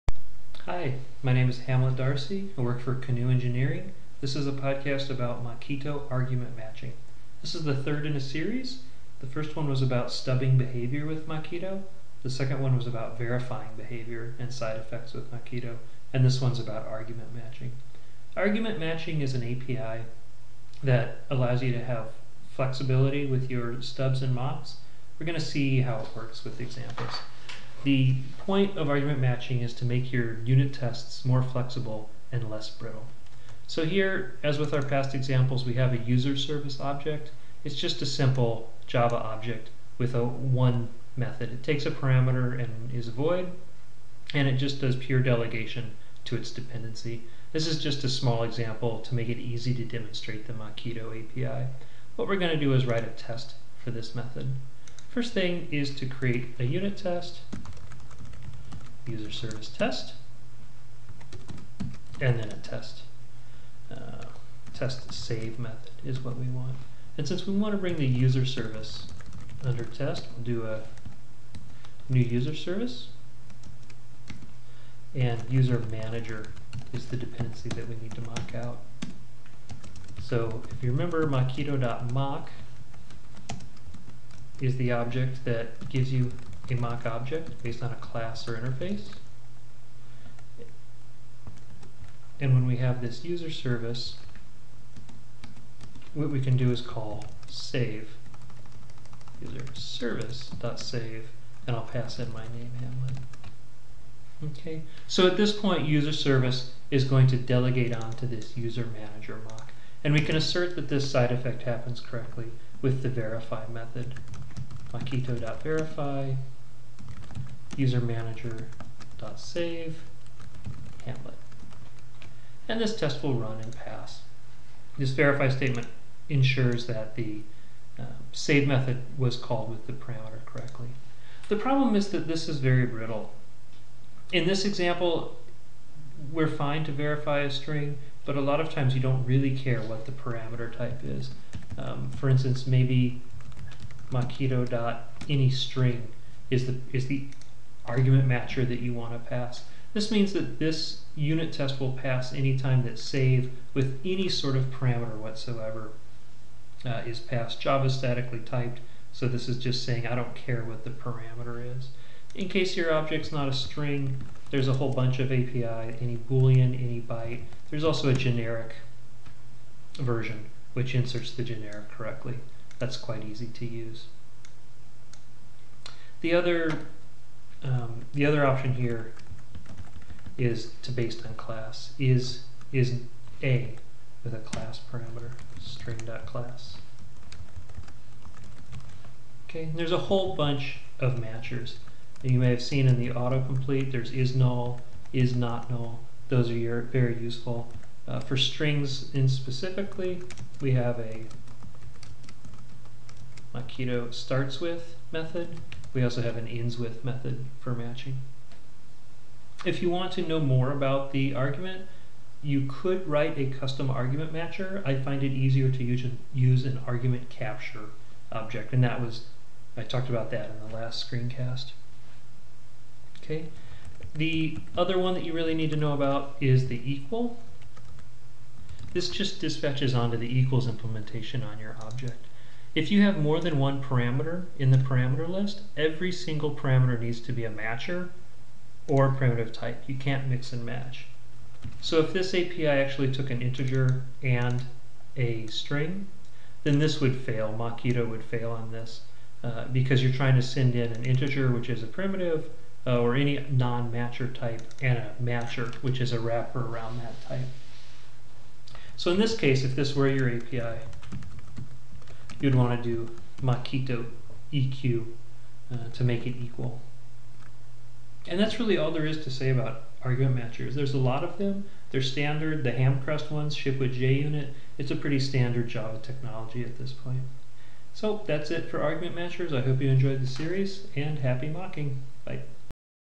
Mockito - Using Argument Matches in Java Unit Tests
A screencast about Java Unit Testing. Shows how to use Mockito Argument Matchers for stubbing and verifying mocks. for the full blog post. to read the Mockito documentation.